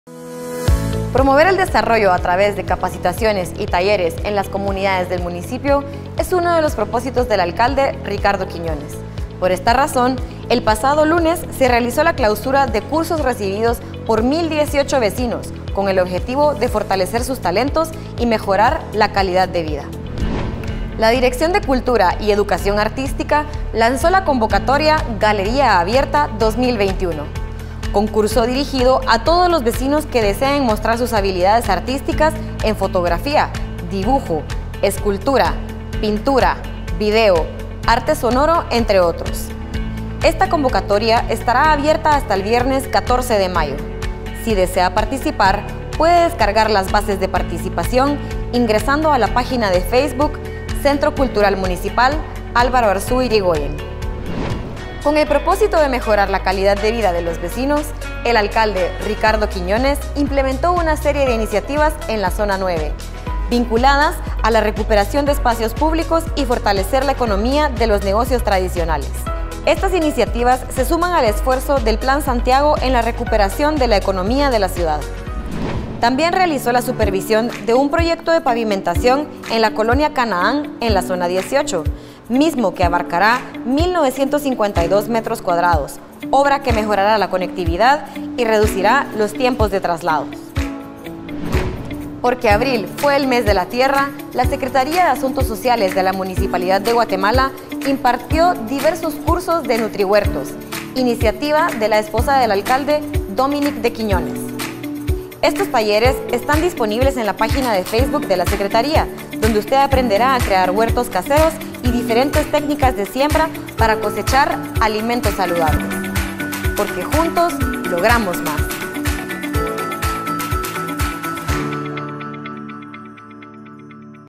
Fortalecer el talento y la calidad de vida
La Municipalidad de Guatemala en la reciente semana, desarrolló varias iniciativas cuyo propósito es fortalecer el talento de los vecinos como la calidad de vida en la ciudad.